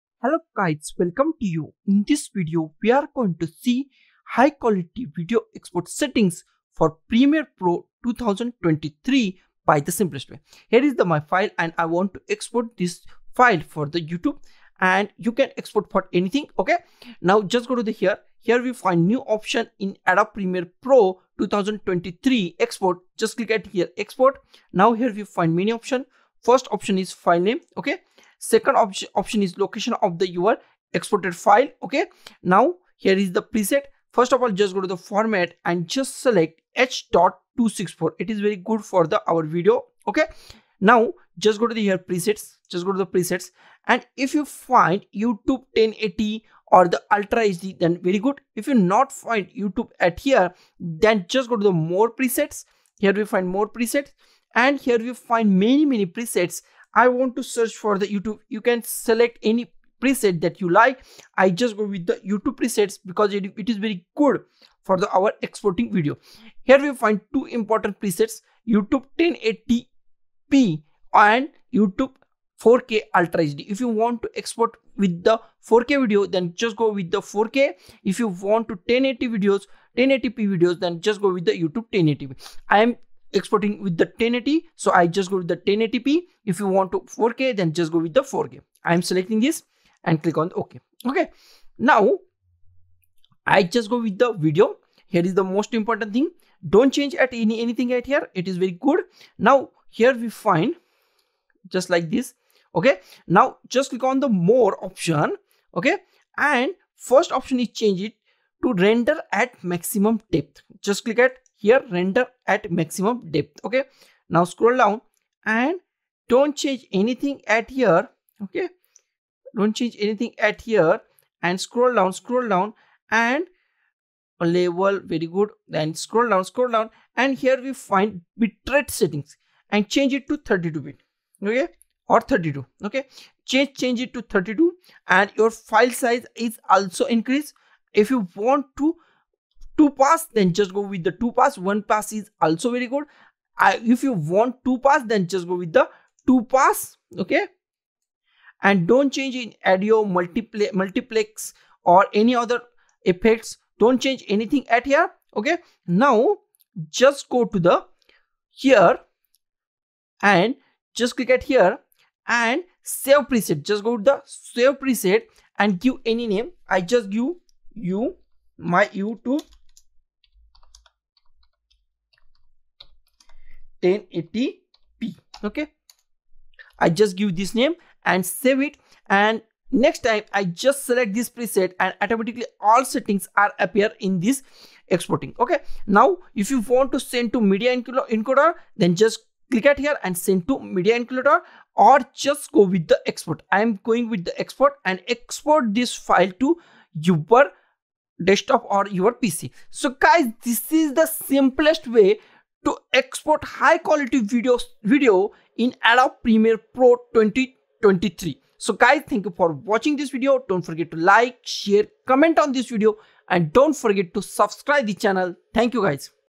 High Quality Video Export Settings For Adobe Premiere Pro 2024
In this Video we are going to see High Quality Video Export Settings For Adobe Premiere Pro 2023. This  Video Settings is good  for  YouTube,  Facebook &  many other  Videos..